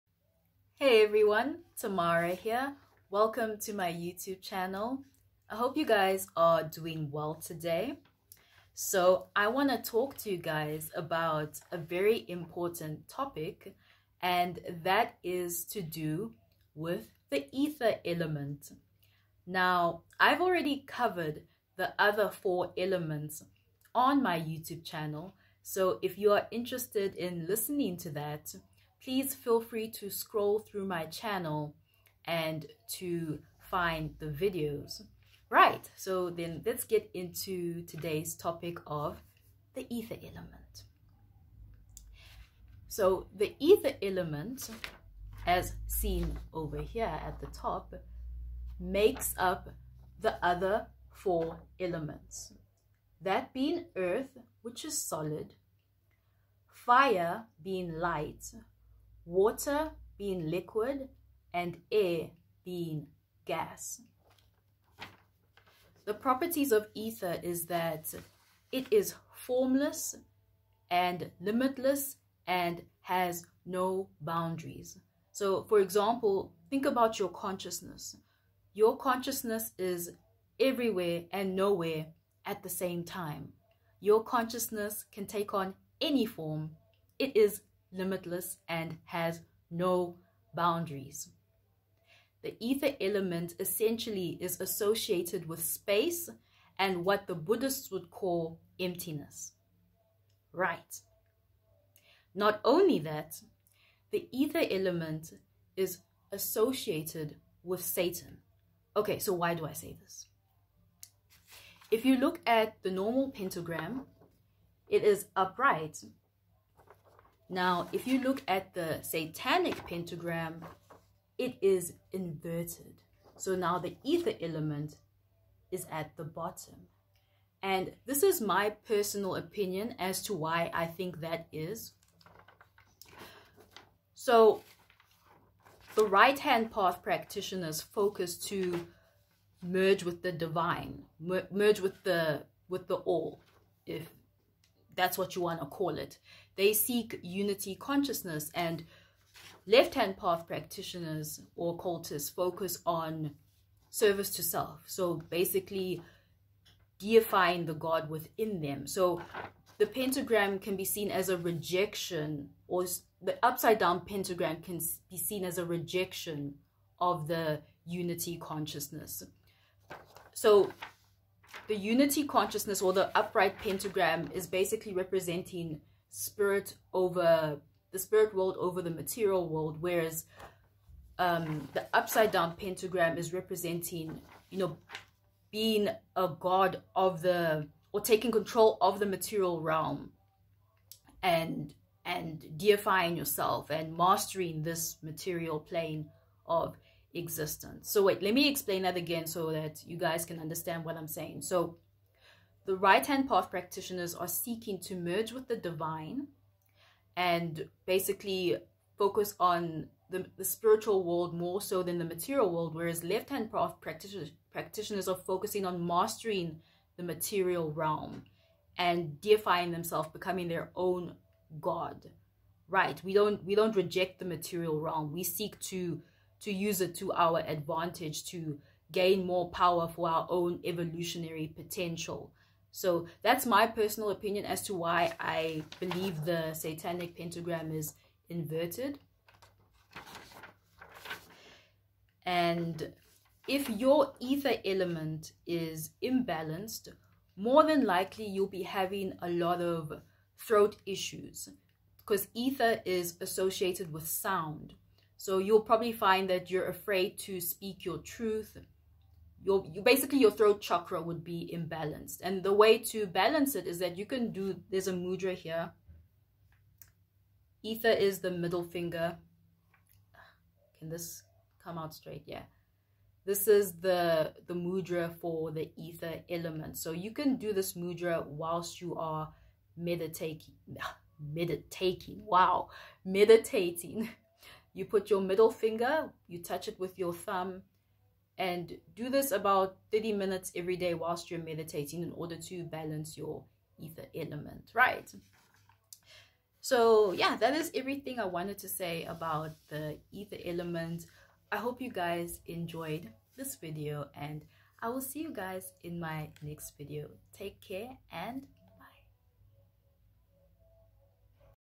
Ether - Spirit Element.
Hey everyone! In this video I give you a short description of the ether element which makes up the other four elements. Enjoy!